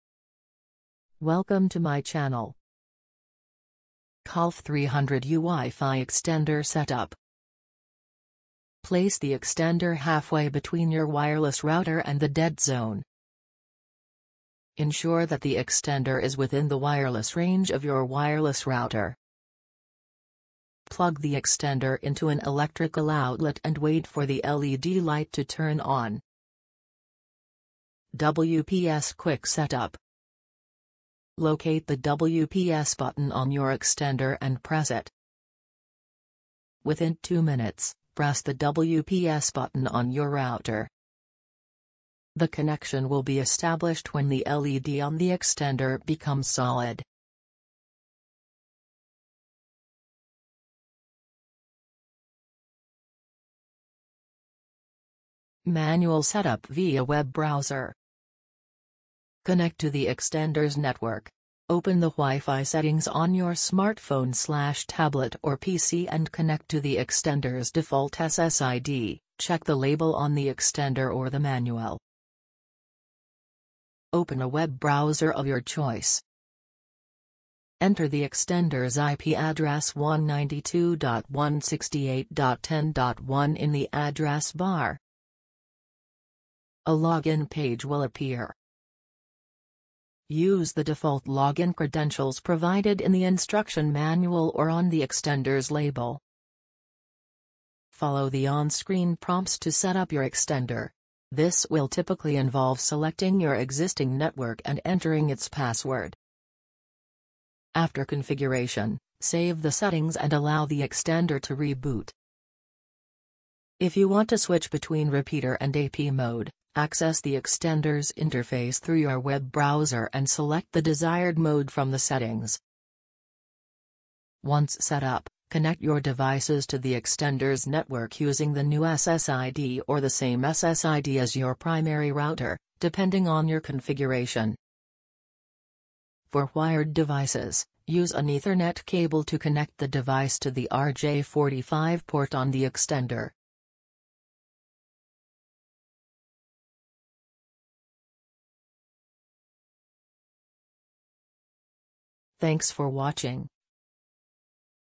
How to Setup Kalff 300u Wifi Extender Signal Booster | Setup Wifi Repeater
Learn the quick and easy steps to set up your Kalff 300u WiFi Extender with our comprehensive guide! Enhance your wireless network coverage and boost signal strength effortlessly. Follow our step-by-step tutorial to ensure a seamless installation process, allowing you to enjoy a stronger and more reliable WiFi connection throughout your home or office. Don't let dead zones limit your internet experience – optimize your network with the Kalff 300u WiFi Extender today!